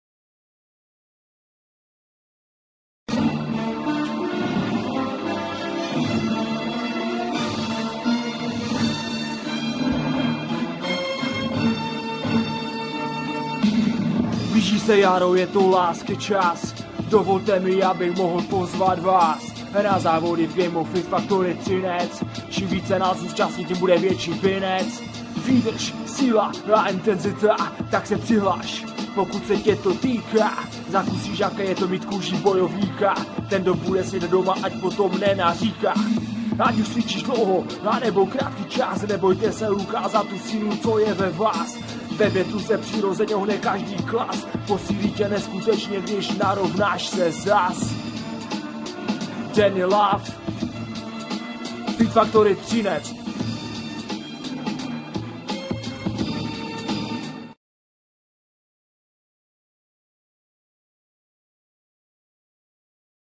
Limitless Spring Tests 2016 - Promo video
Promo video na jarní závody Limitless Spring Tests, pořádané Fit Factory Třinec

text: Blíží se jaro je to lásky čas,
         dovolte mi abych mohl pozvat Vás,
         na závody v Gymu Fit Factory Třinec,
         čím víc se nás zúčastní tím bude větší binec.
         Výdrž, síla a intenzita, tak se přihlaš,
         pokud se Tě to týká, zakusíš jaké je to mít kůži bojovníka,
         ten kdo bude sedět doma ať potom nenaříká.
         Ať už cvičíš dlouho a nebo krátký čas,
         nebojte se ukázat tu sílu co je ve Vás,
         ve větru se přirozeně ohne každý klas,
         posílí Tě neskutečně, když narovnáš se zas.